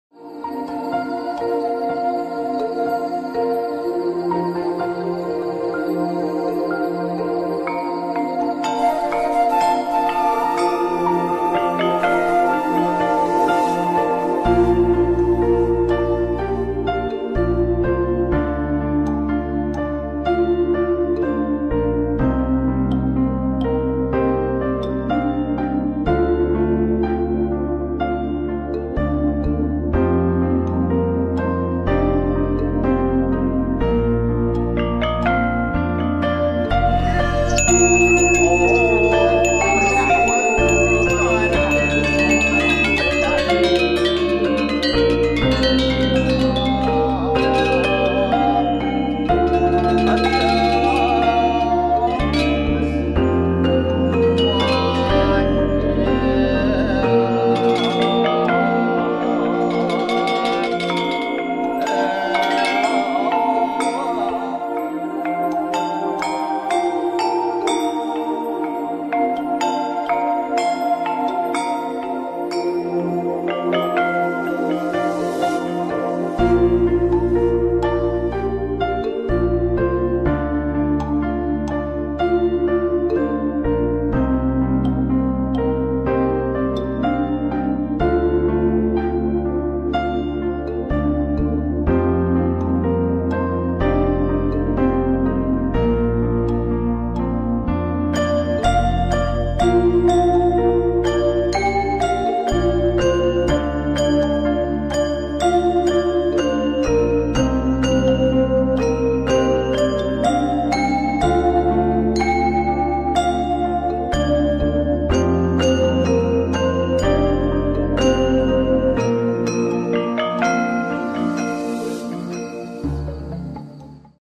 3 Bulanan | Cinematic Video | a6300 | SEL35mmF18
Helo Good people

this is one of several cultures in Bali which is called as
"metelu bulnanan"